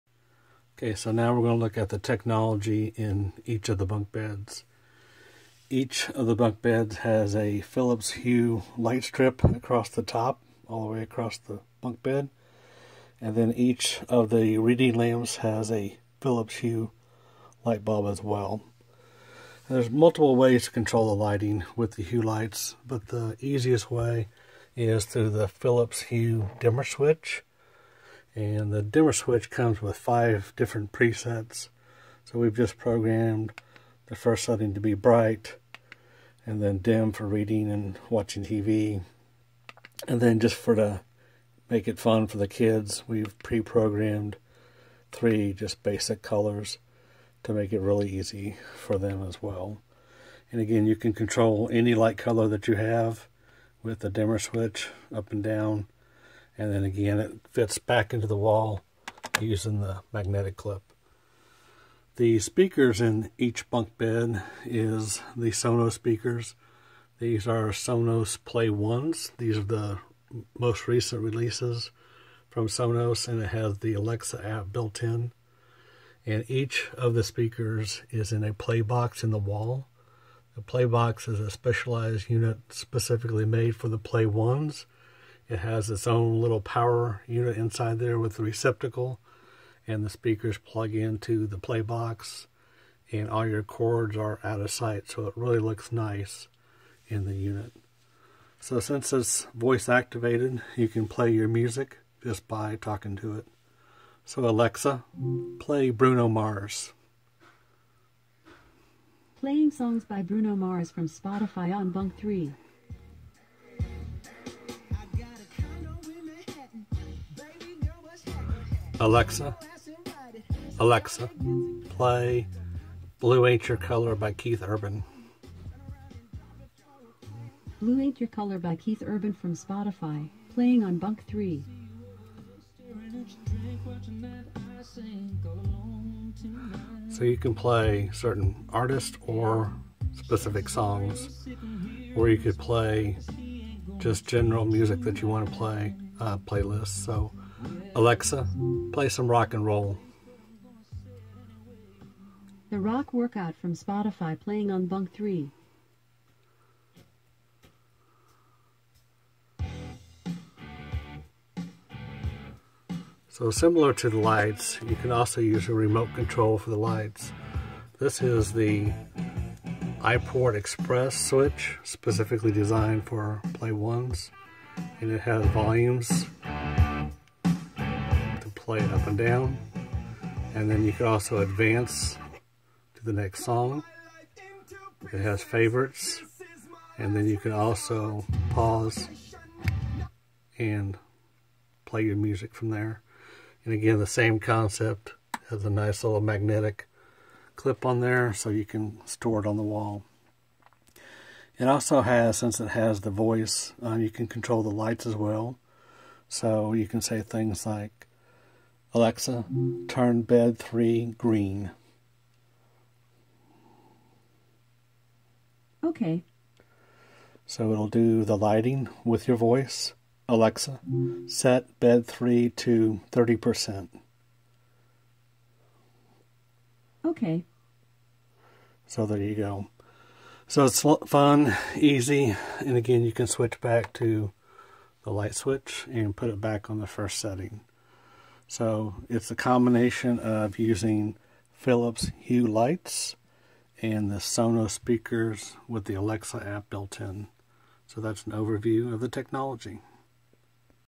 Bunk Bed Project Technology
Bunk bed project with Philip Hues lights, Sonos speakers, Iport Xpress and Playbox